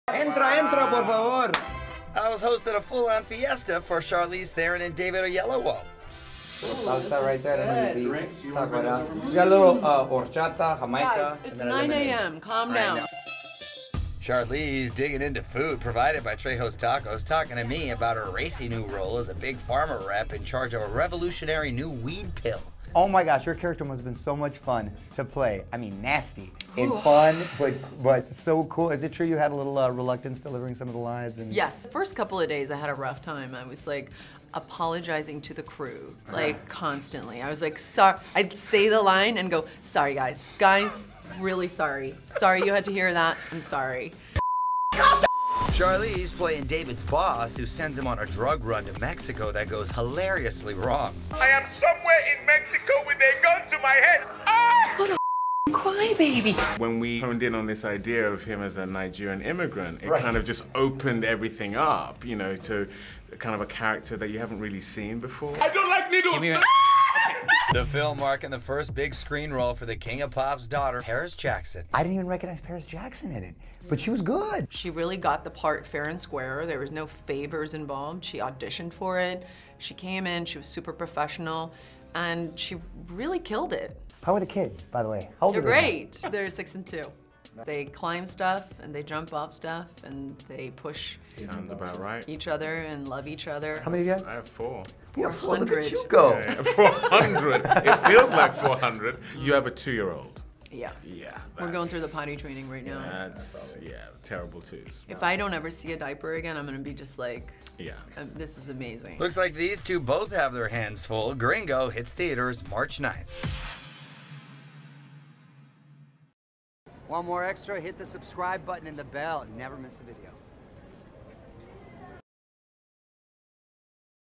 Charlize Theron Dishes on Daughter August’s Big Milestone
“Extra’s” Mario Lopez sat down with Charlize Theron and David Oyelowo as they promoted their film “Gringo.”

Theron also gave an update on her two kids, Jackson, 6, and August, 2. She smiled, dishing, “They’re good. They climb stuff and jump off stuff. They push each other and love each other.”

August is in the middle of “potty training.” Theron admitted, “If I don’t see another diaper again, I’m going to be like... ‘this is amazing.’”

“Gringo” hits theaters March 9.